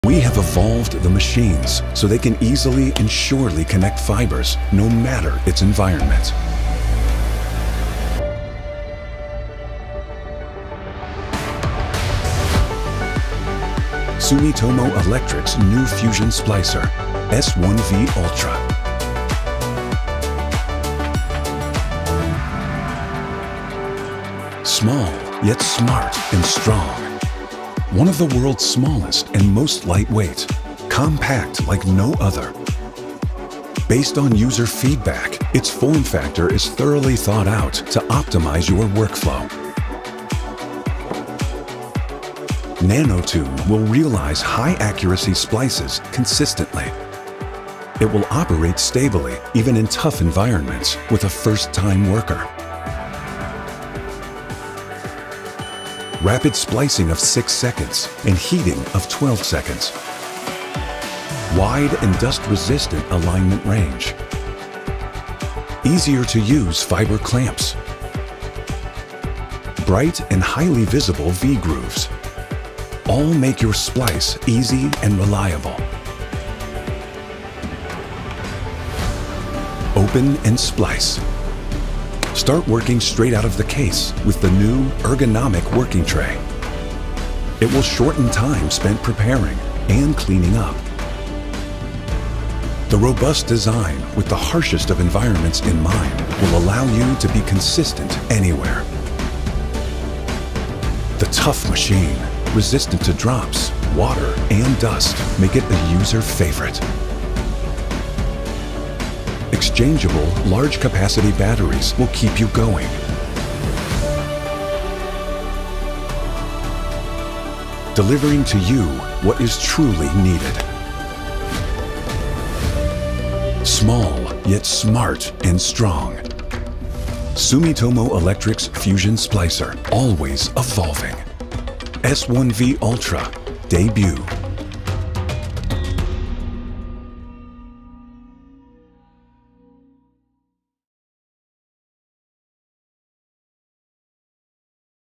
NEW SPLICING MACHINE!! Sumitomo S1V Ultra - Made in Japan
New Launch! 🎉 Introducing the Sumitomo S1V Ultra – in India. 🇮🇳✨ With Sumitomo's Latest FTTx Innovation Experience a new dimension of precision and efficiency with the future of splicing technology. New S1VUltra - Small, Smart and Strong.

Get the best splicingmachine brand in India.

Find the S1V Ultra fusionsplicer specification and download pdf on our